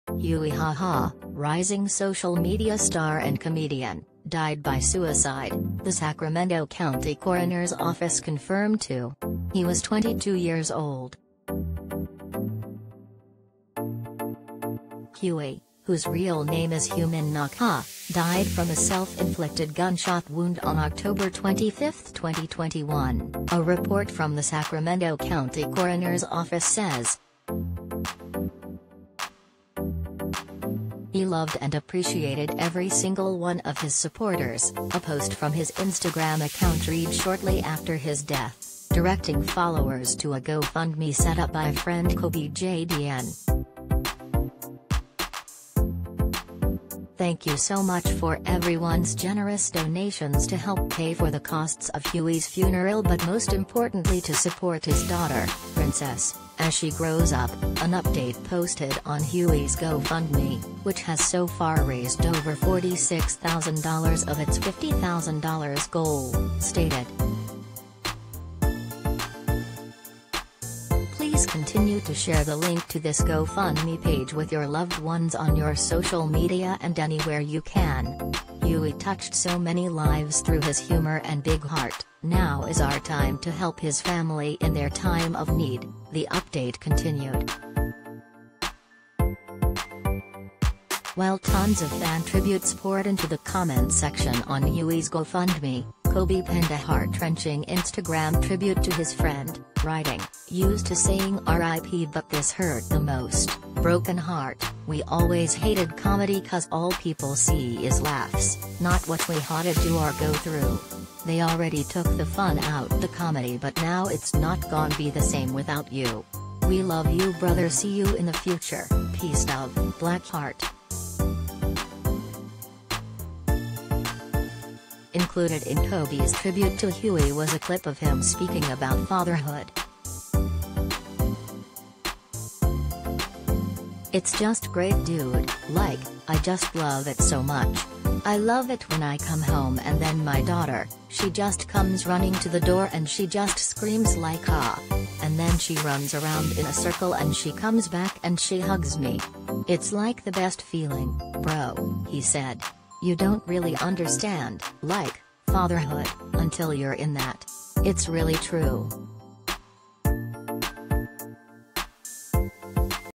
Huey Haha, Rising TikTok Star and Comedian, Died by Suicide: Coroner
Huey Haha, rising social media star and comedian, died by suicide, the Sacramento County Coroner's Office confirmed to . He was 22 years old.
Huey, whose real name is Hieu Minh Ngoc Ha, died from a self-inflicted gunshot wound on Oct. 25, 2021, a report from the Sacramento County Coroner's Office says.
"He loved and appreciated every single one of his supporters," a post from his Instagram account read shortly after his death, directing followers to a GoFundMe set up by friend Coby Jdn.